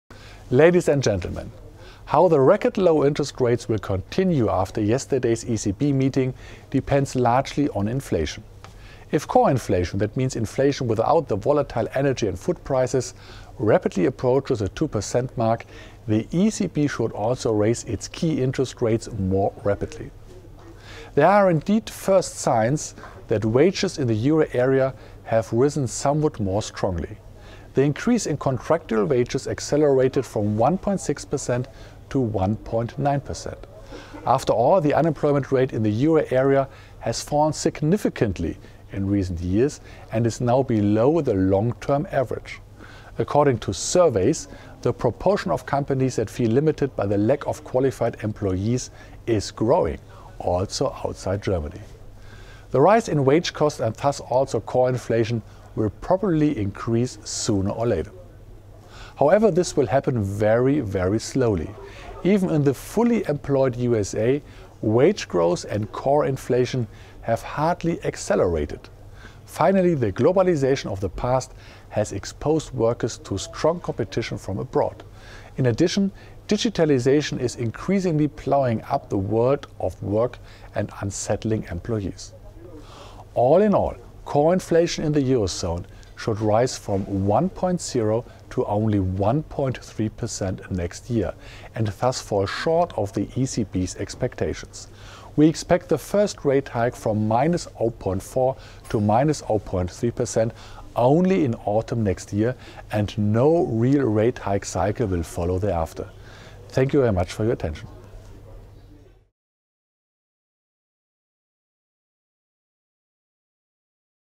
Euro area: Inflation will pick up, but only slowly
15 June 2018: At yesterday’s press conference, the ECB demonstrated its conviction that over the medium term euro zone inflation will rise towards its 2% target. In fact, labour shortages together with heightened inflation expectations suggest higher wages, and consequently consumer prices.

For more information please refer to the web site of our Chief Economist